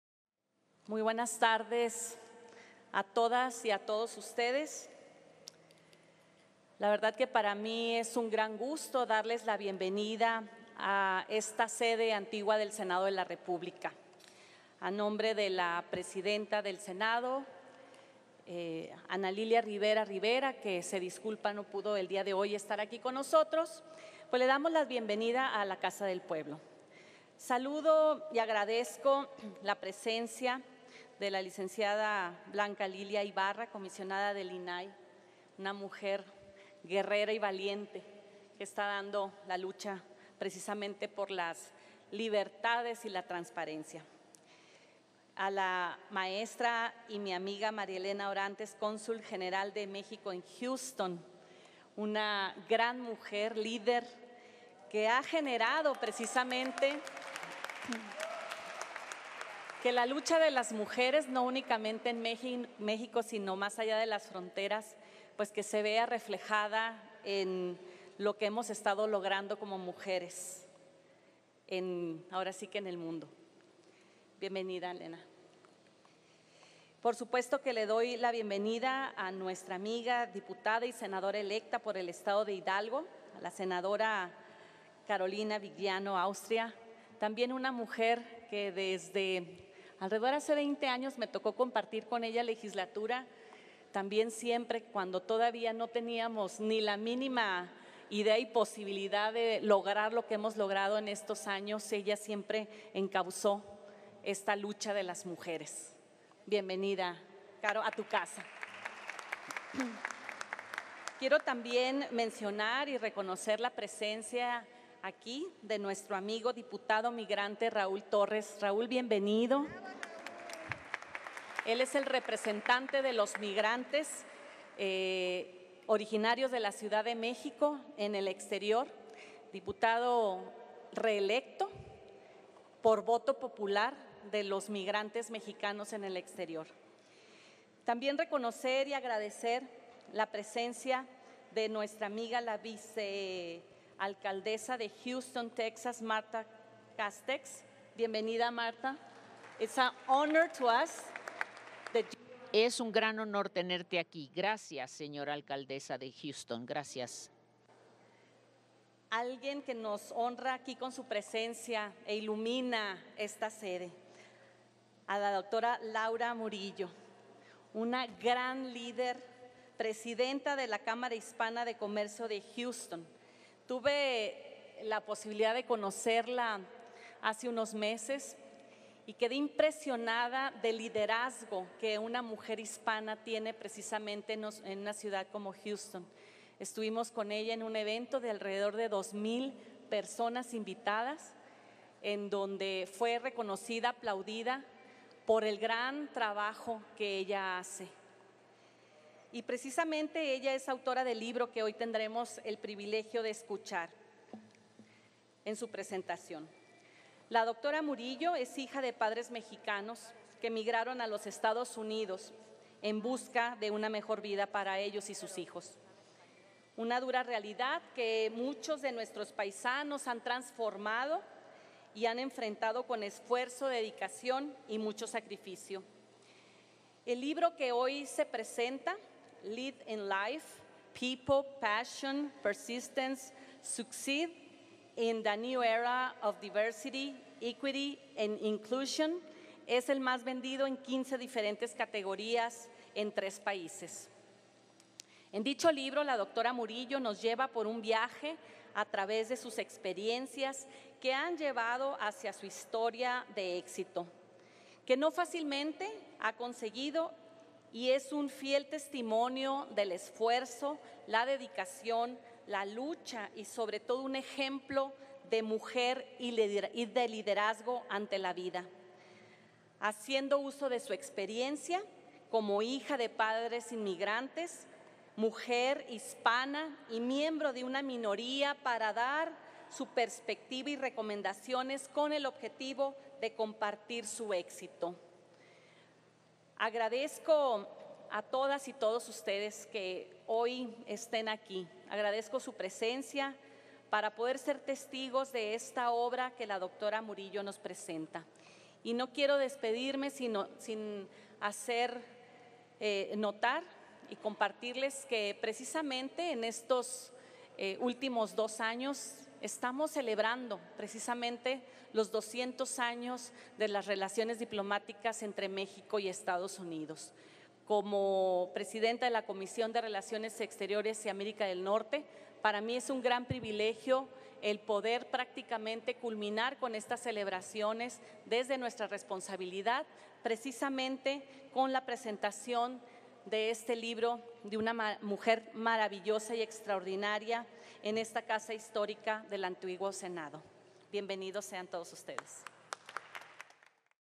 Senadora Gina Andrea Cruz Blackledge, en la presentación del libro “Lead in life. People. Passion...
11 de julio de 2024.- intervención de la senadora Gina Andrea Cruz Blackledge, en la presentación del libro “Lead in life. People. Passion. Persistence: succeed in the new era of diversity, equity, and inclusion”.